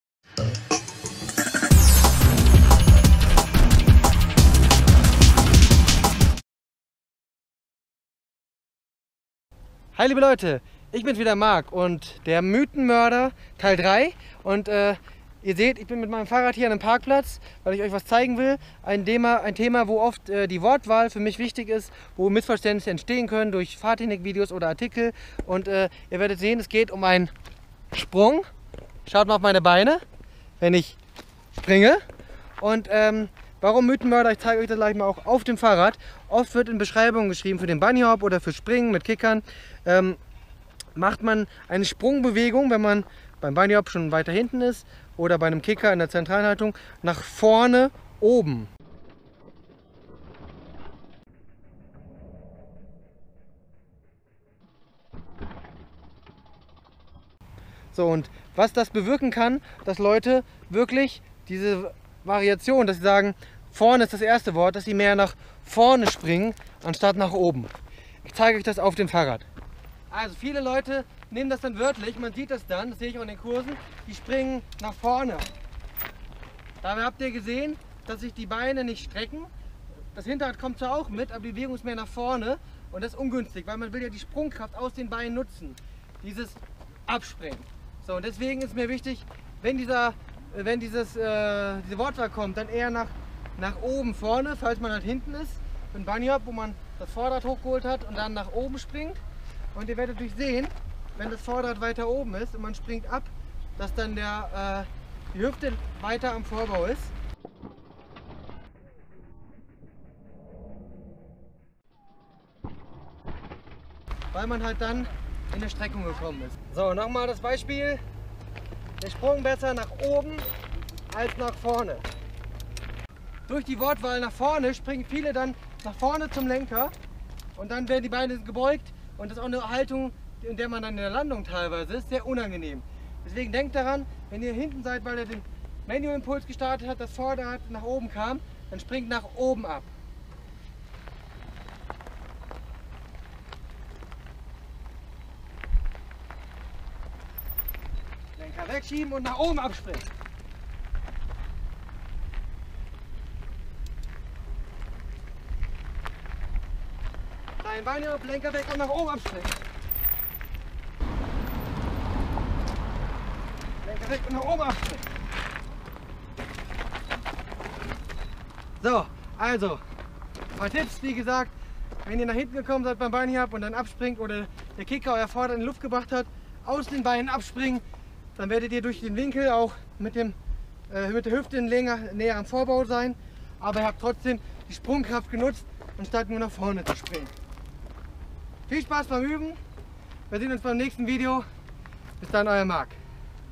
Mythen Mörder #3 - Absprung mit Vorne-Oben-Impuls?
Häufig liest man (oder sieht es in Videos), wie jemand beschreibt man sollte für das Hinterrad-Anlupfen nach "Vorne-Oben" abspringen - diese Wortwahl bringt Nachteile mit sich, in diesem Video erkläre ich das kurz!